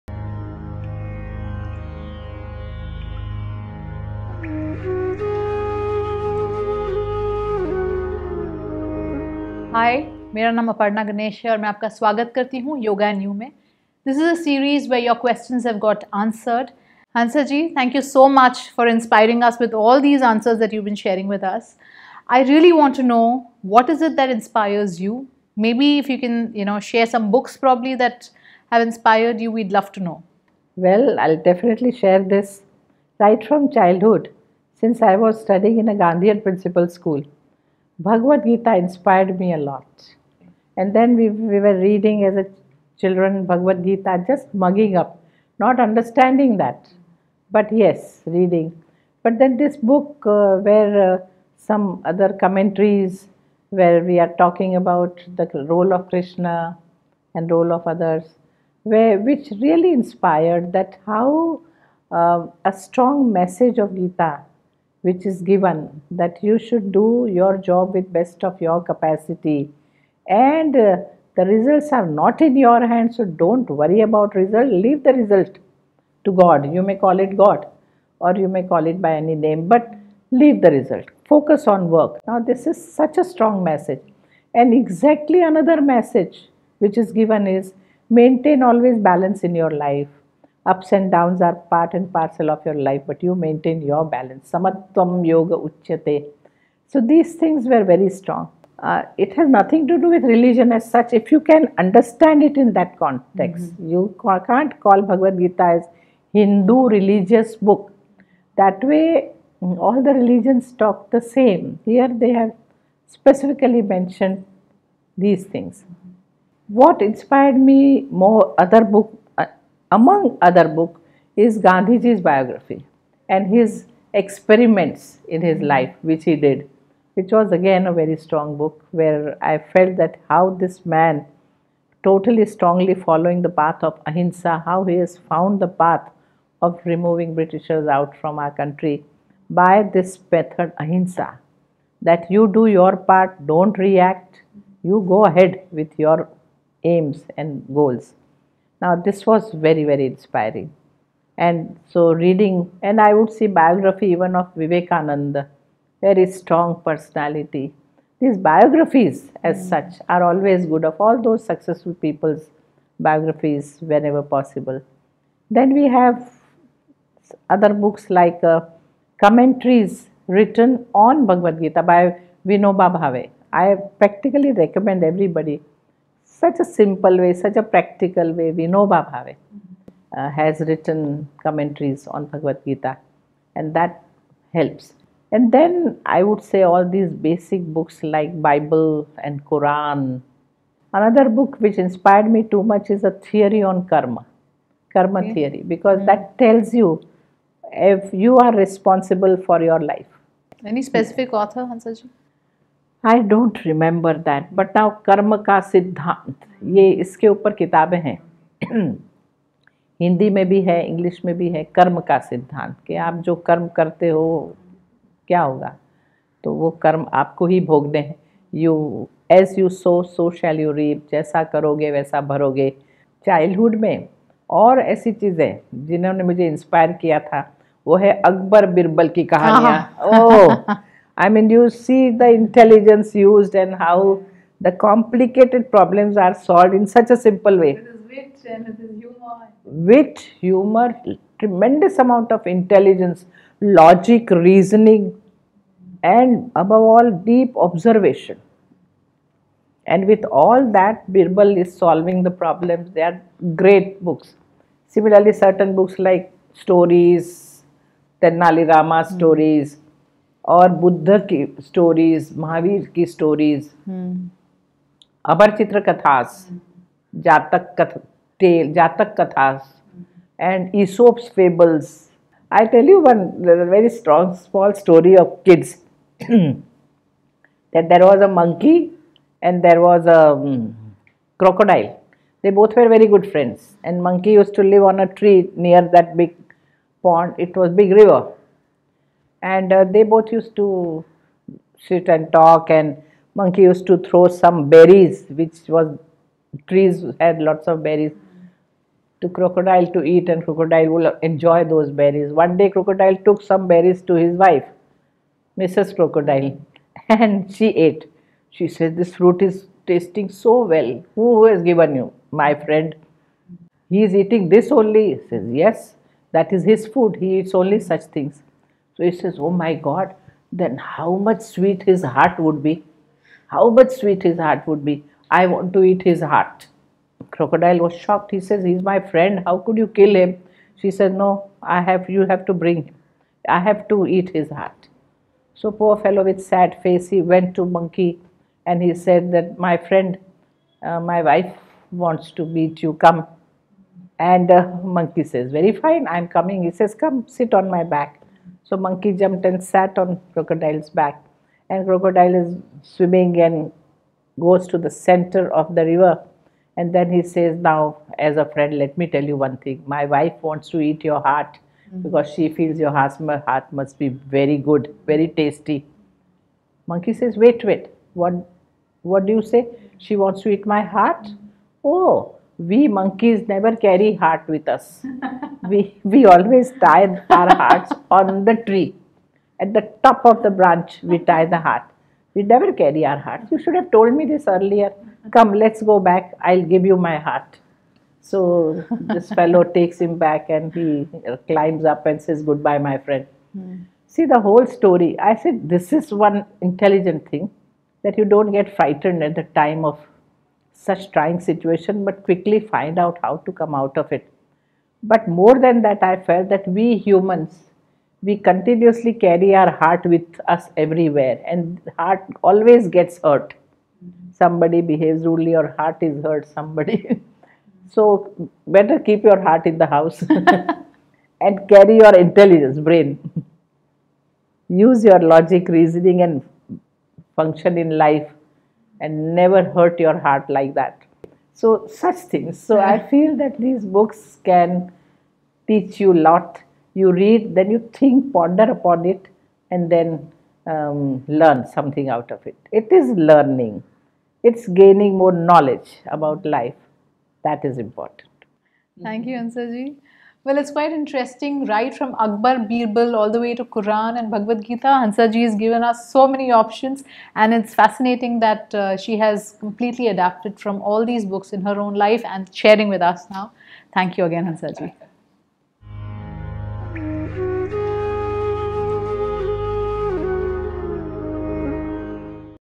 Yoga & You: Must read books that inspire us | Dr. Hansaji Yogendra
here are books that lead us to a better understanding of a way of living and life. Dr. Hansaji talks about these books and tells how they can inspire us. Watch this video and make these books part of your life to achieve a healthy body, mind and soul.